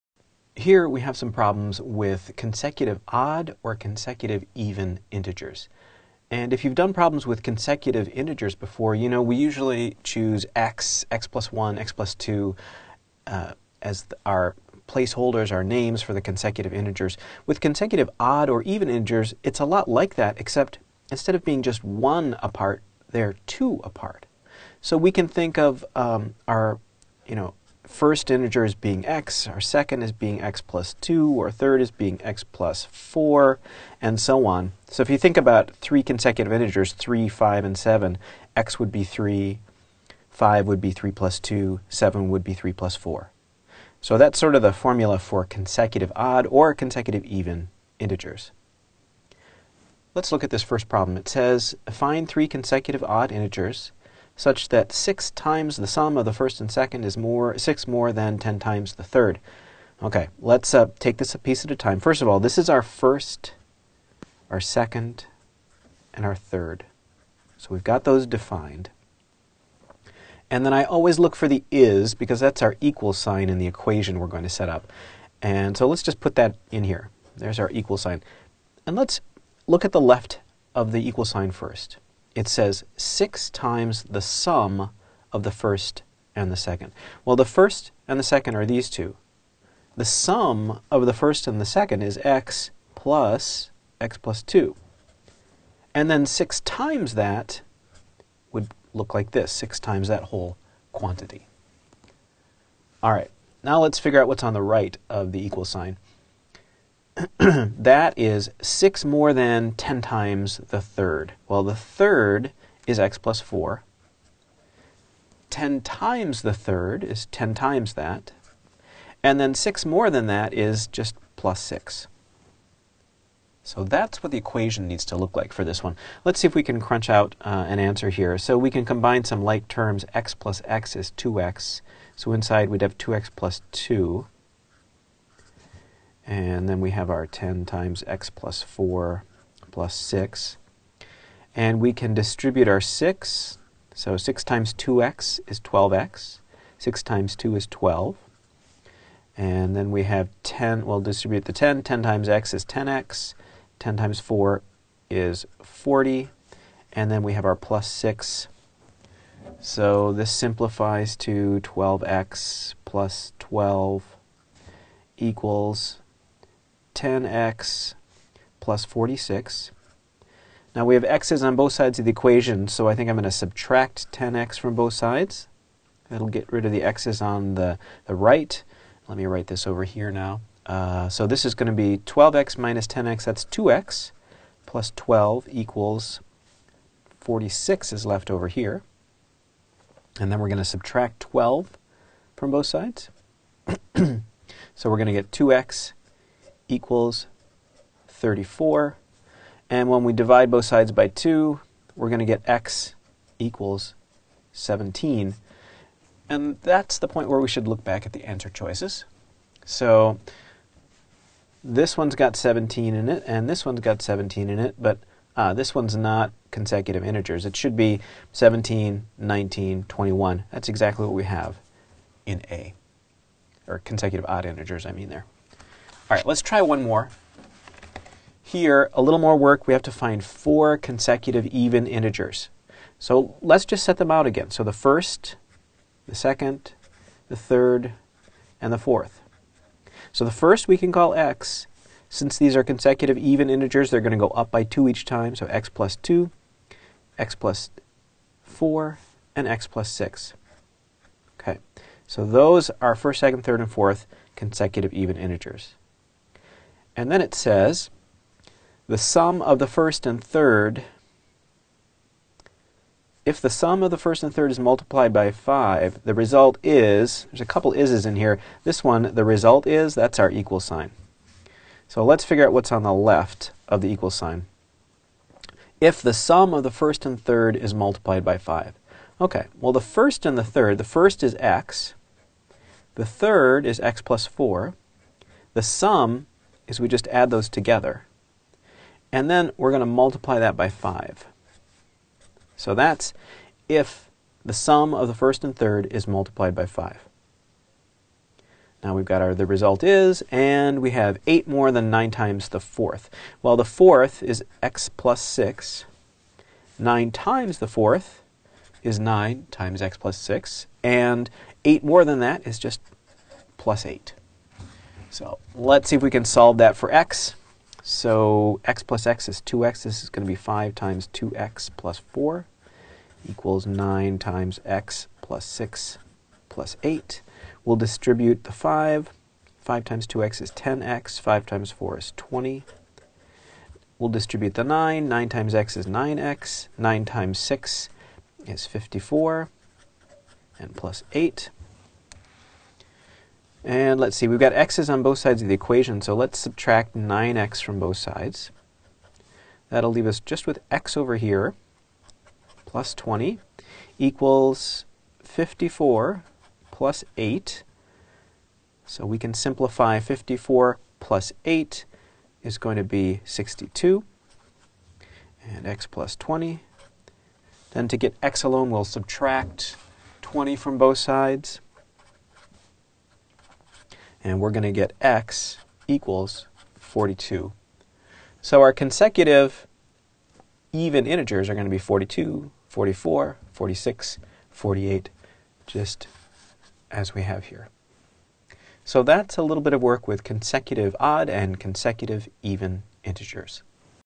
Consecutive Odd Integer and Consecutive Even Integer Word Problems
How to solve word problems about consecutive odd and consecutive even integers